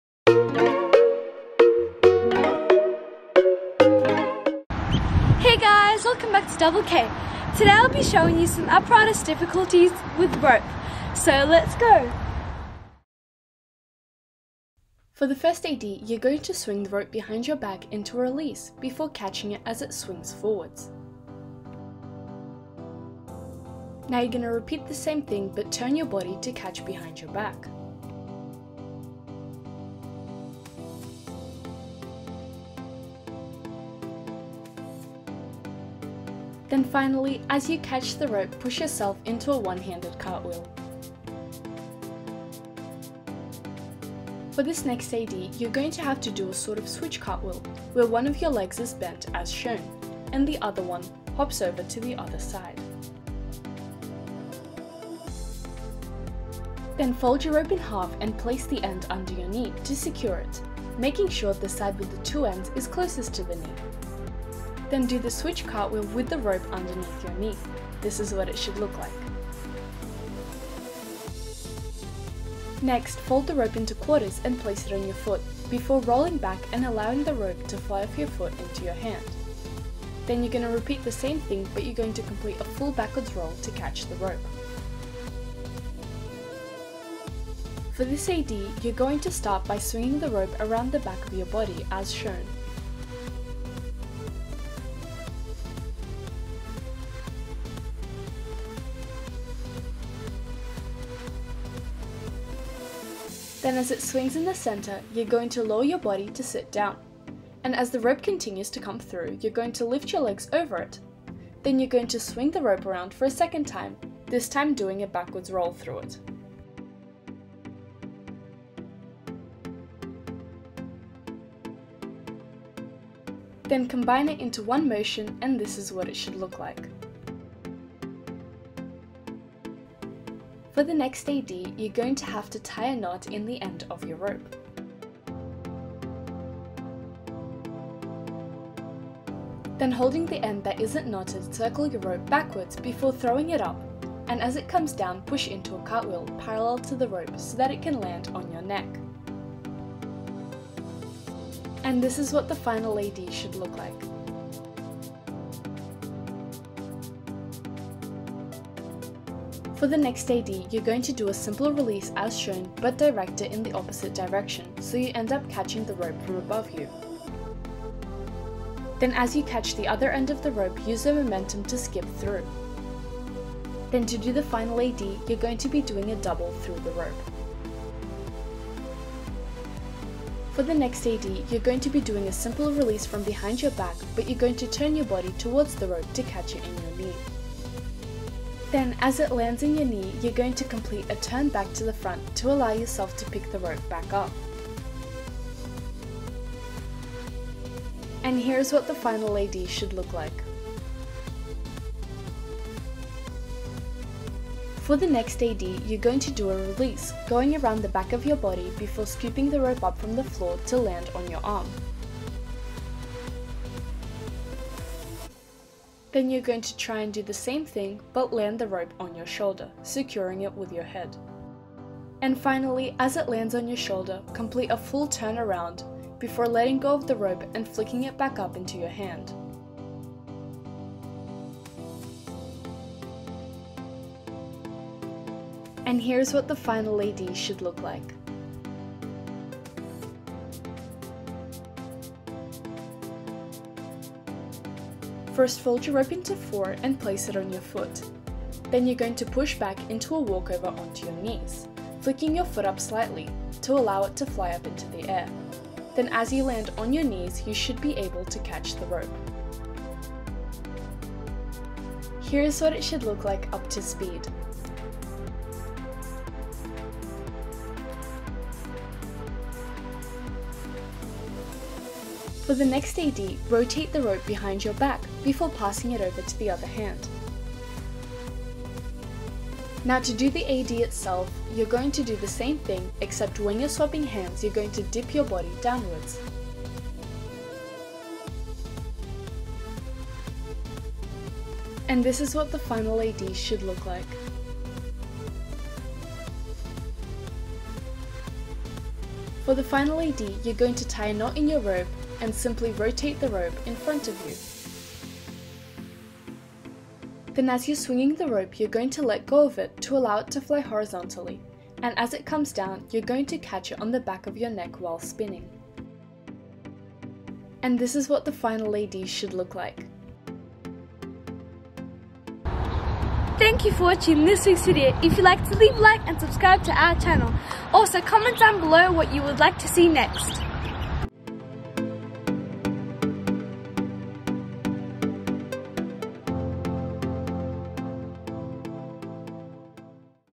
Simple Apparatus Difficulties with Rope | Rhythmic Gymnastics
In this video I will be showing you step by step how to do some apparatus difficulties with rope. Hope you enjoy :)